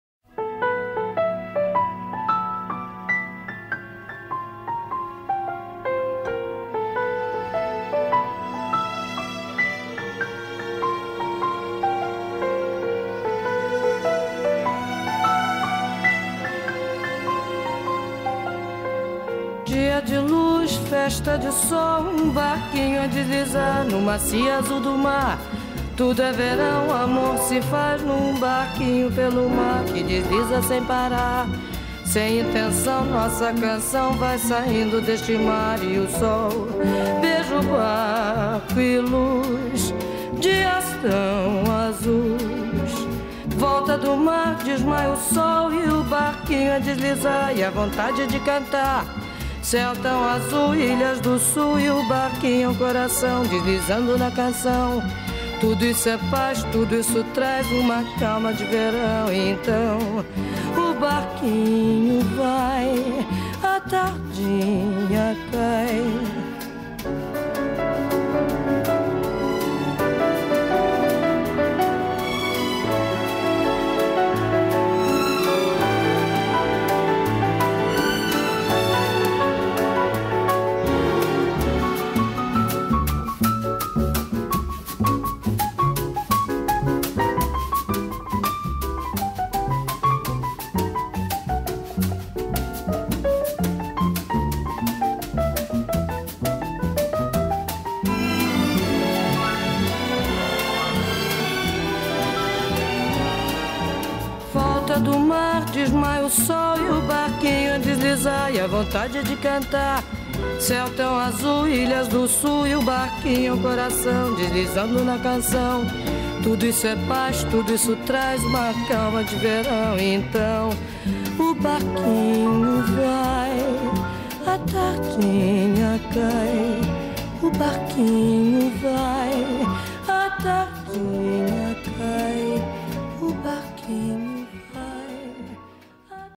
Making of João Dias Verão 2011
Nesta Coleção Verão 2011, sugerimos uma regata pelos 7 mares com destino a terras tropicais.

Uma paleta de azuis, turquesa, petróleo, celeste flúor ou marinho, nosso preto desta coleção, as clássicas listras em vermelho e branco, cambraias de linho e voiles com estampas de espumas do mar, ou ainda crepes de malhas com borboletas surreais e imagens paradisíacas